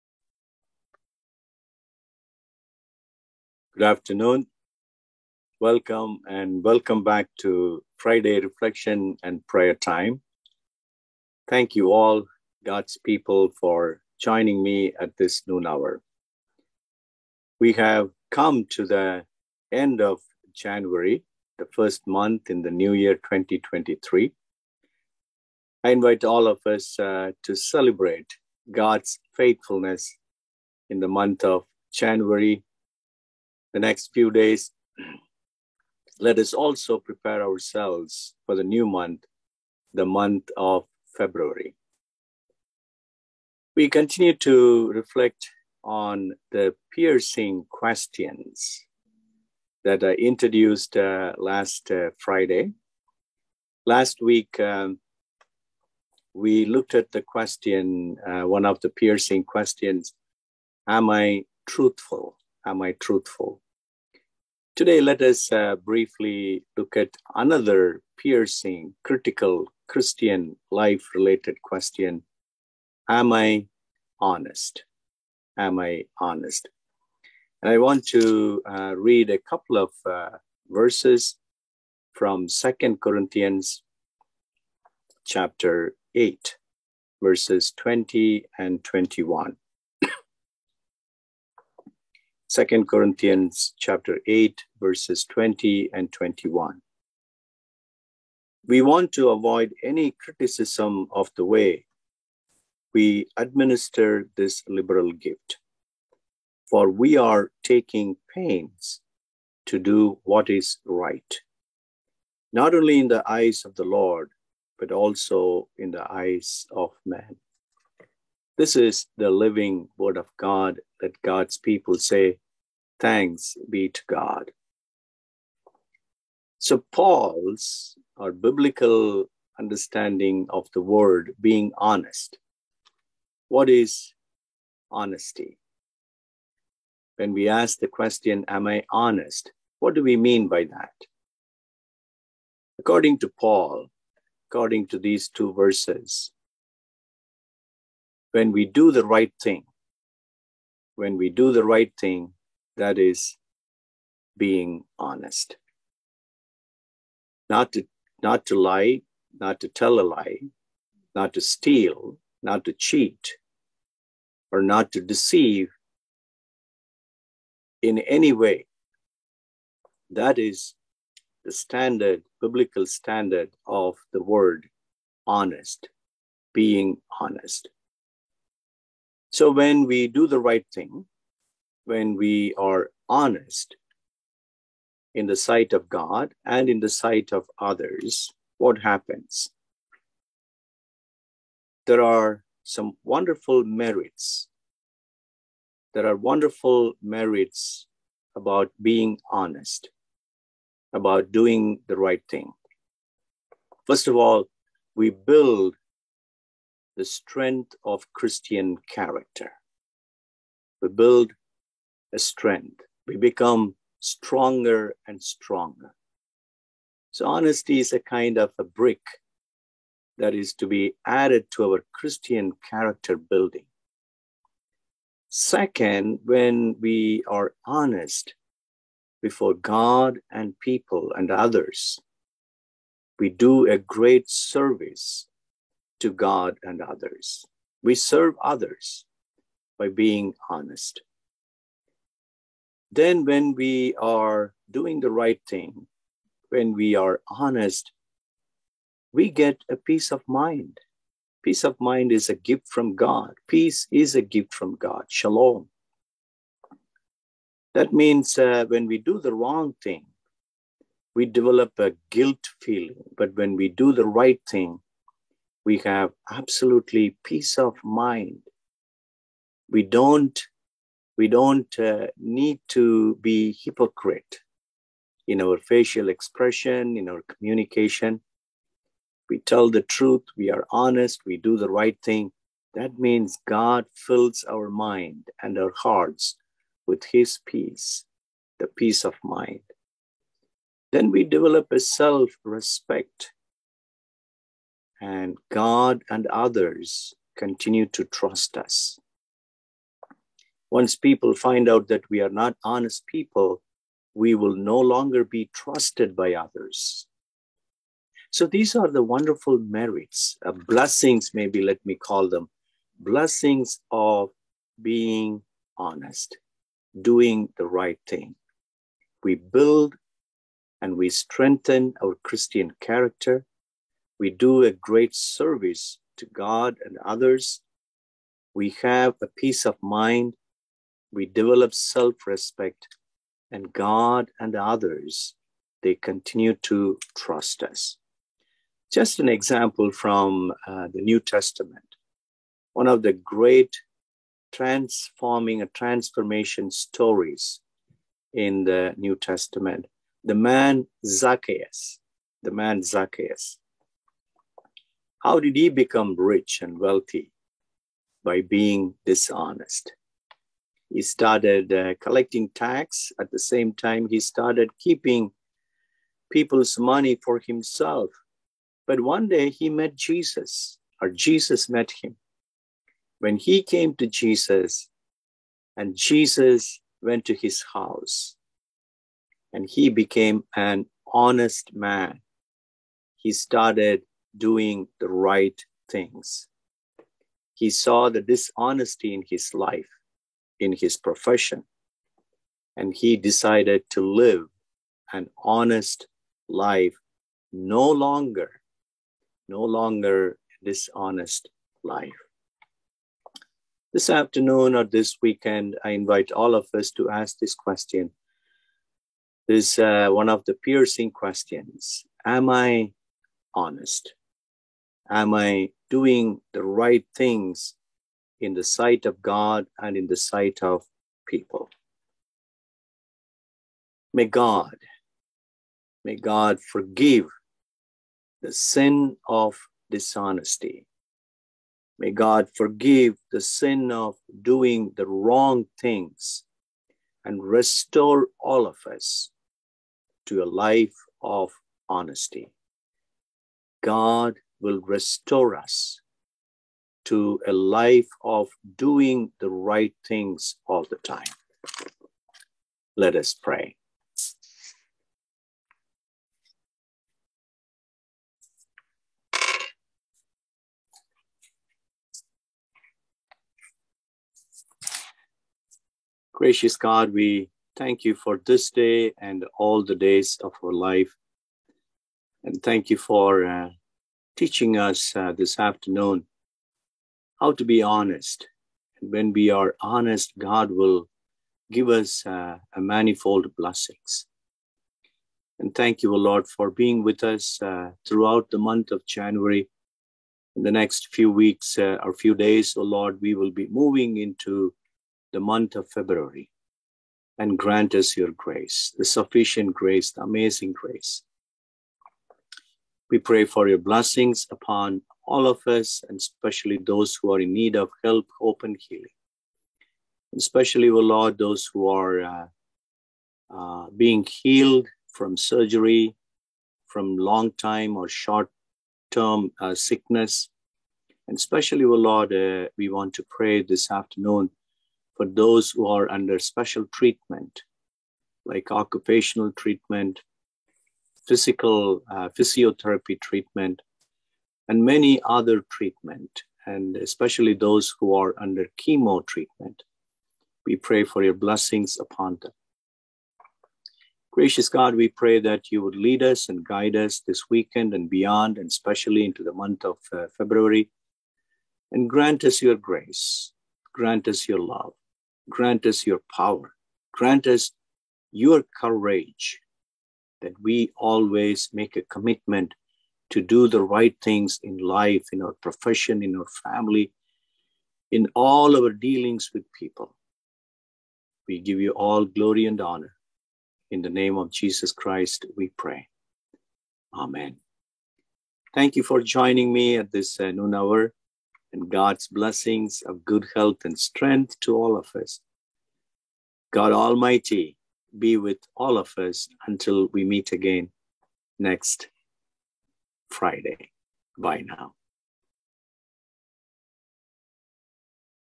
Reflection and Prayer, January 27, 2023, FUMCWA
First United Methodist Church West Allis, WI, Reflection and Prayer, Sam Royappa